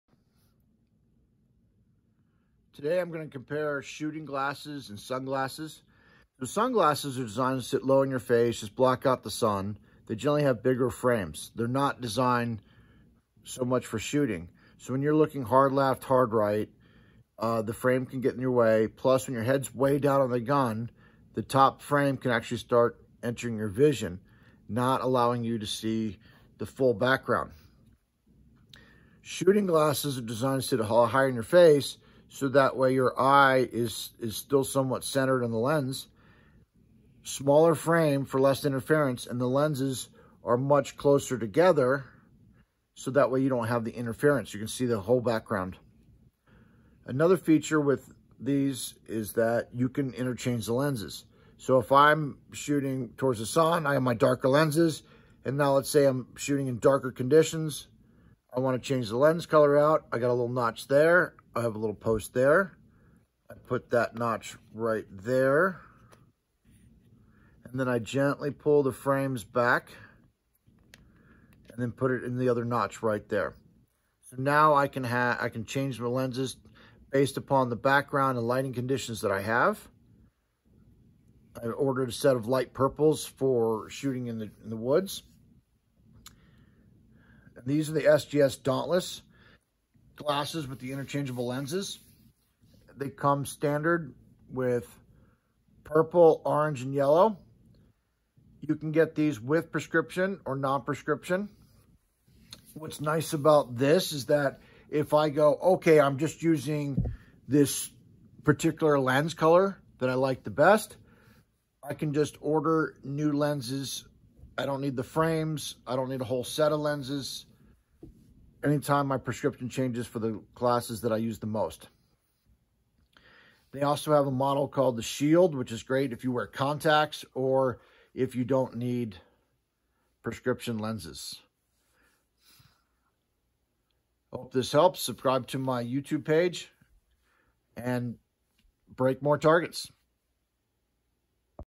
Why Shooting glasses are better than sunglasses.
This is a review of SGS shooting glasses verses sunglasses. These are the SGS dauntless shooting glasses with interchangeable lenses.

DeanBlanchard.com